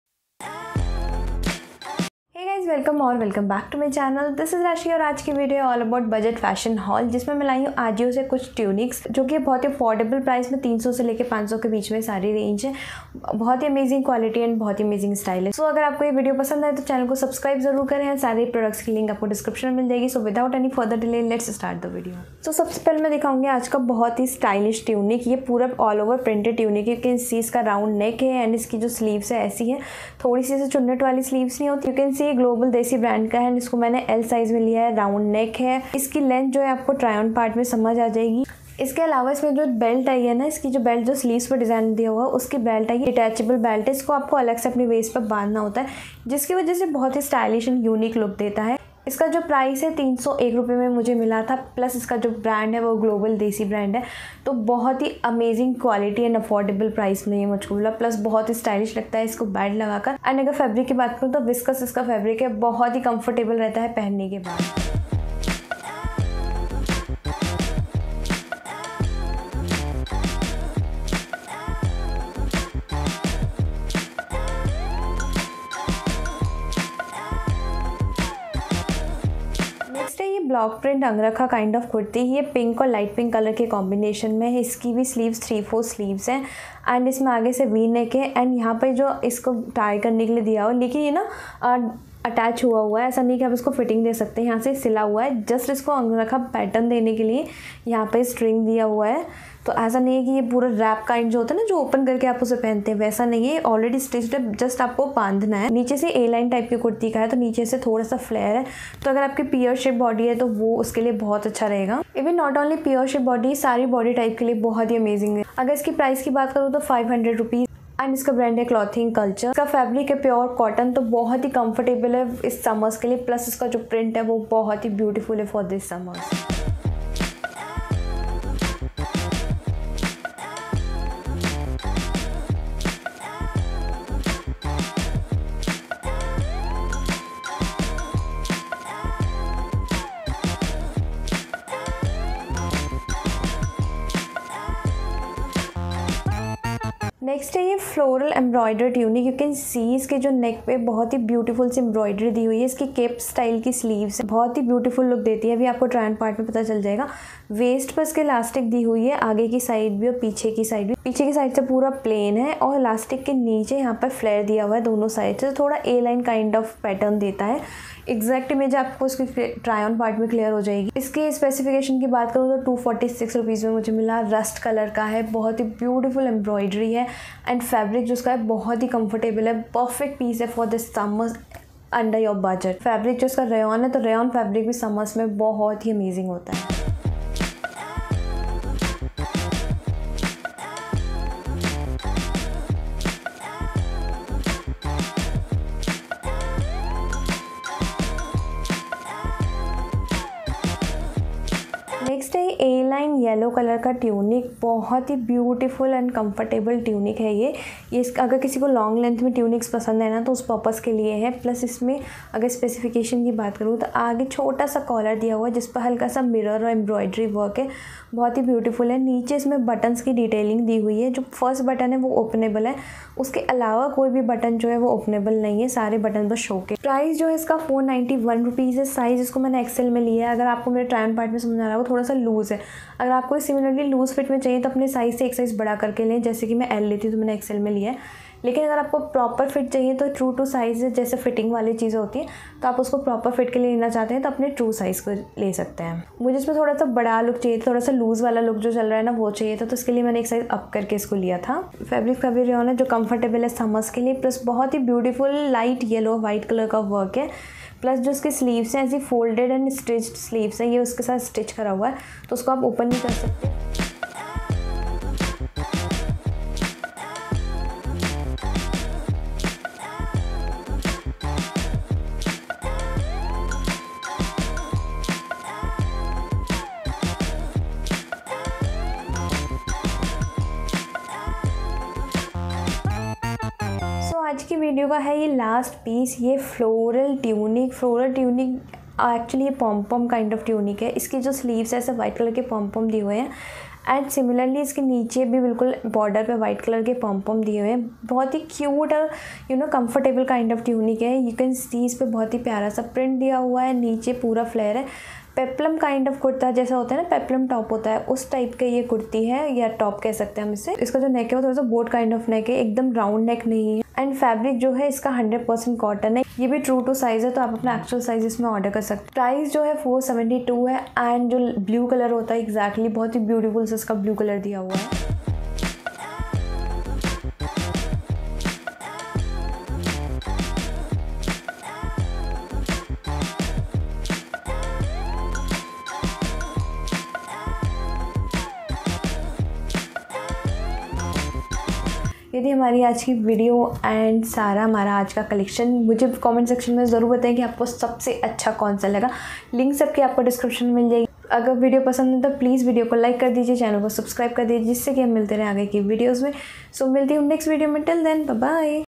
Best affordable Ajio Tunic Haul Under 300 - 500/- | Ajio Short Kurti Haul | Ajio long top Haul
Hello fam,
स्वागत है आपका मेरे चैनल "Lifestyle Updates By Rashi" मे I इस वीडियो में मैंने शेयर किया है ajio tunic haul for this summer season । Love Love !
Outfit Links:

5. Blue Pom pom tunic